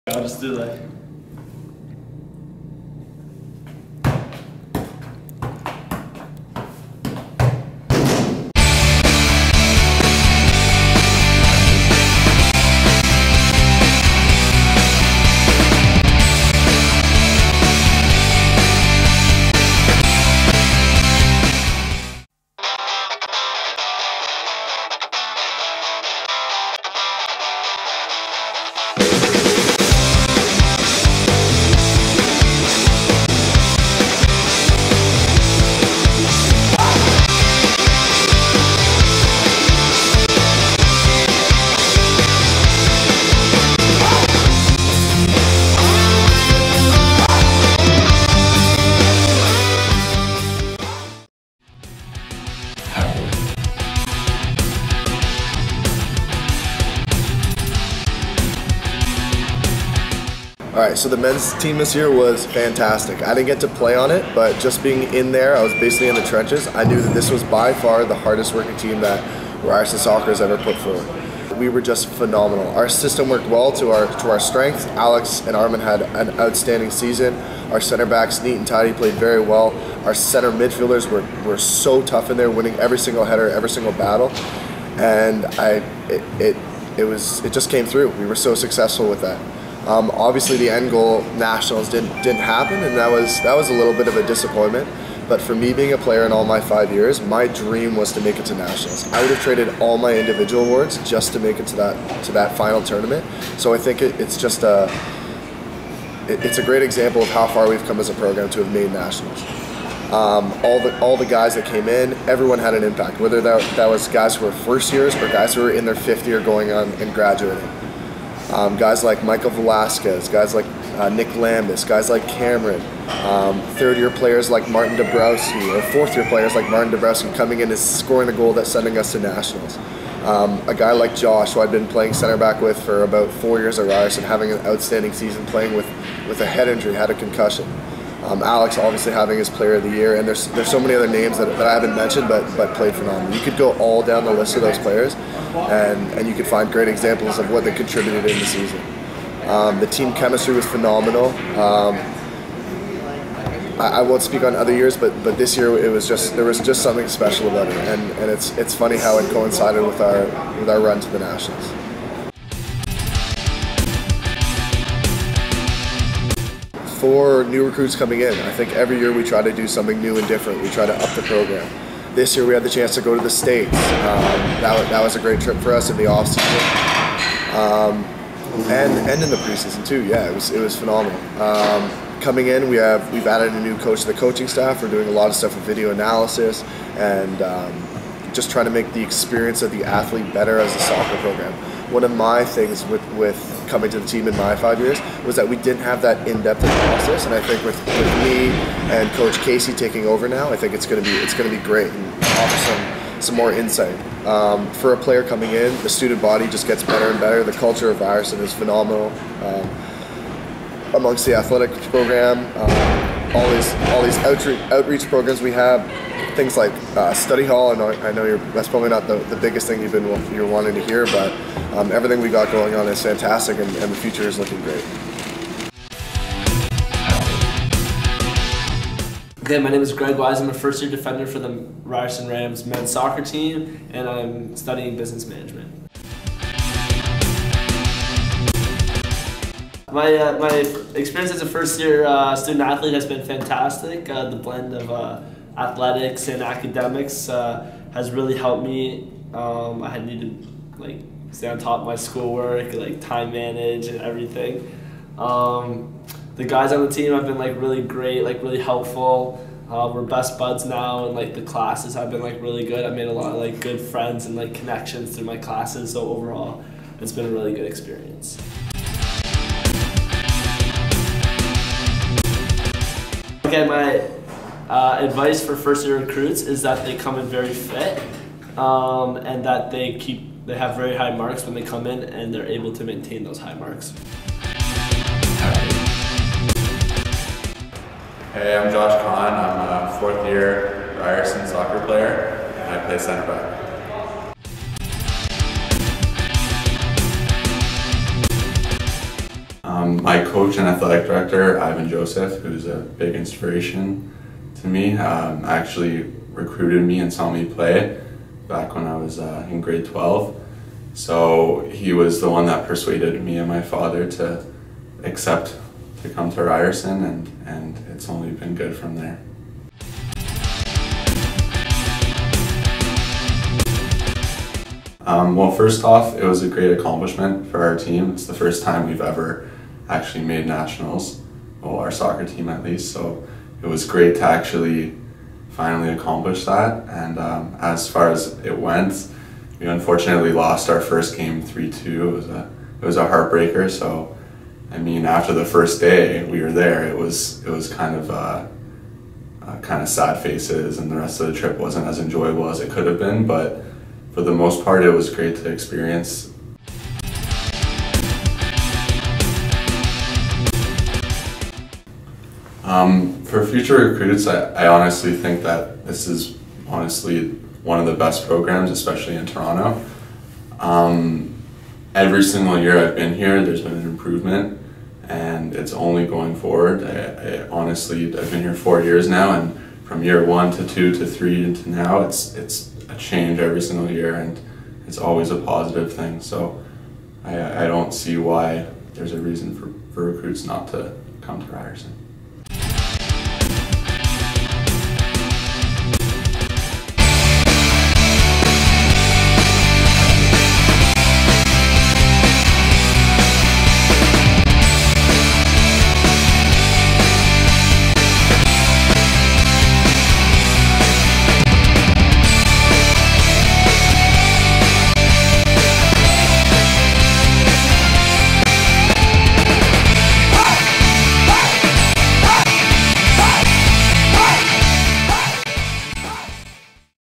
Ryerson Rams Episode Two: MensSoccer
A look at the Ryerson Rams men's soccer team, with interviews with the team's assistant coach and two players.